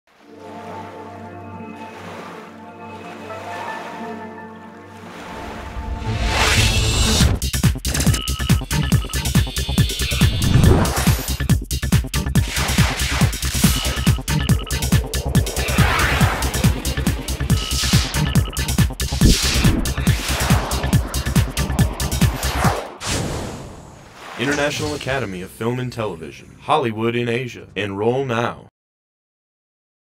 060303 IAFT Bottle PAL.mov
A 2006 commercial - a guy finds a 'genie' in a bottle. The genie is IAFT, who helps make his dreams come true.